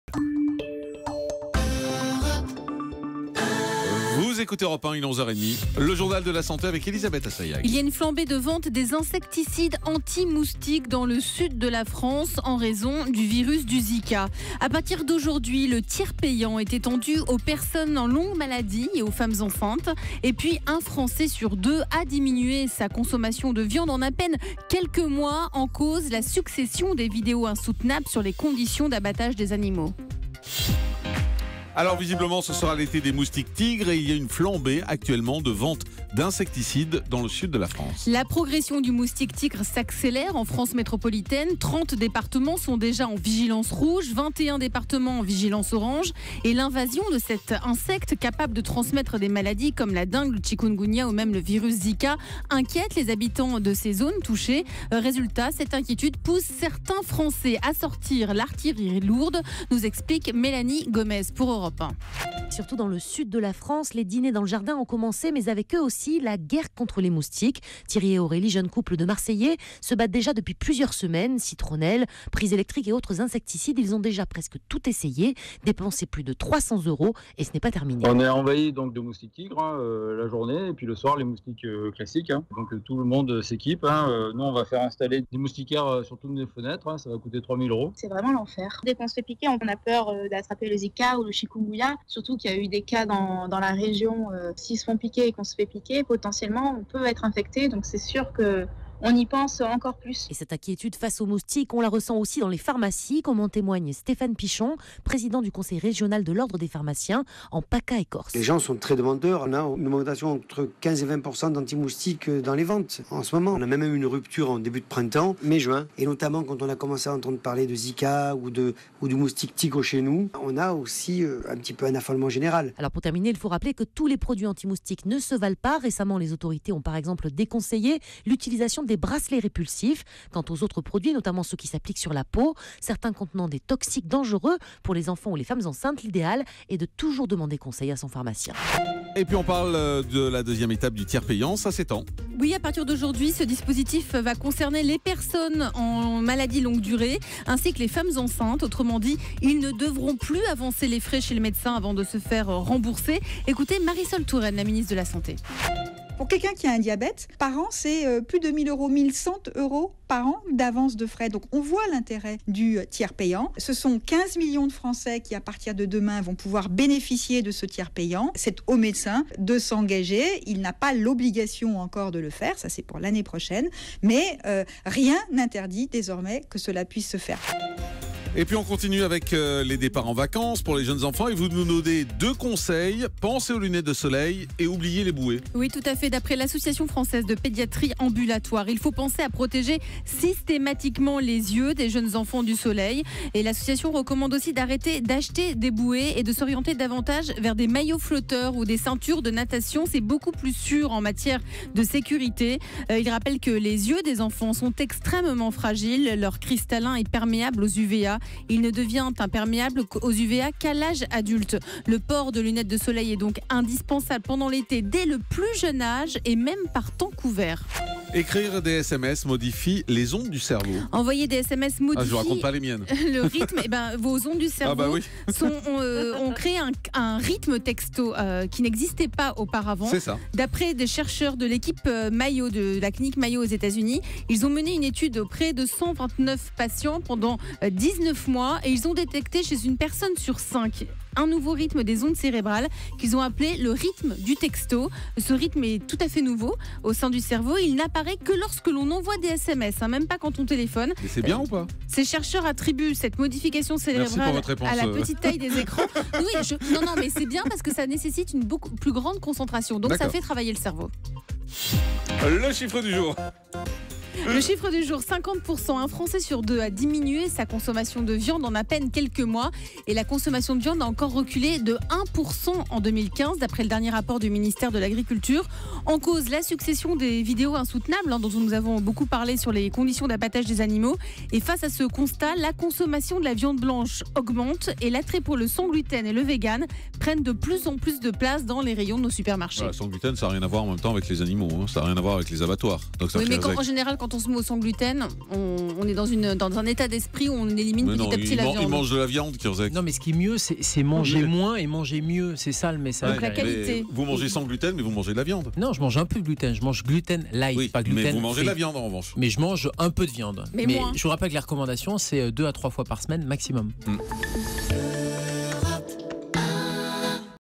Virus Zyka : les ventes d'insecticides en forte hausse dans le sud
Les ventes d'insecticides explosent dans le sud de la France, notamment à cause du virus Zyka.